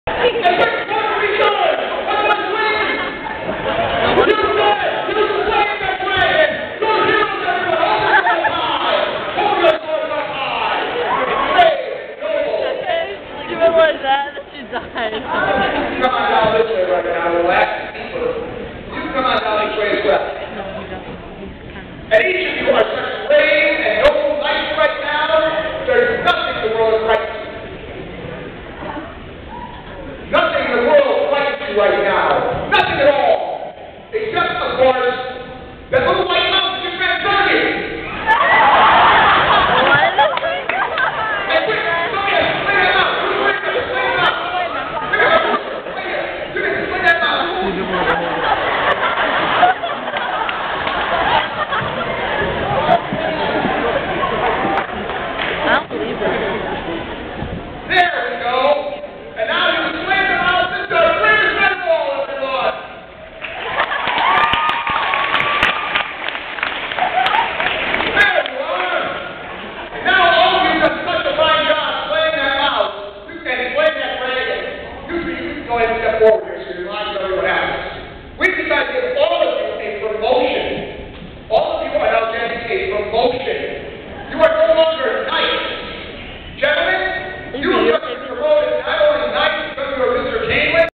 Hypnotist at Kaneland
So this hypnotist came to our school...and hypnotized some guyz at our school.